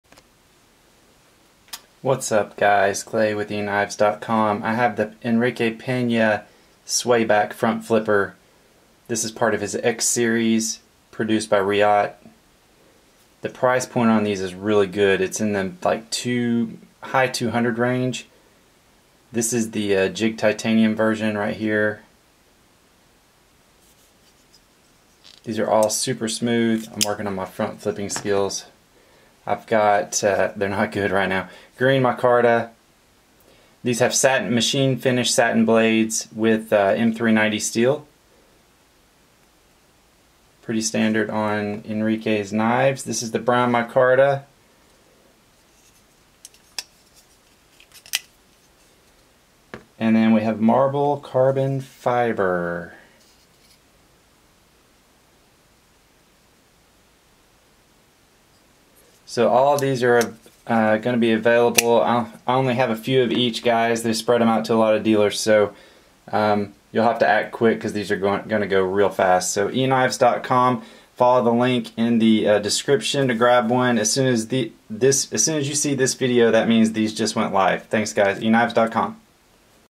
Enrique Pena "Swayback' X-Series Front Flipper Knife
The long anticipated Swayback front flipper production knife from Enrique Pena is here. 4 flavors to choose from including Brown and Green Micarta, Jigged Titanium and Marble Carbon Fiber. All come with Titanium hardware and ceramic bearing for smooth flipping.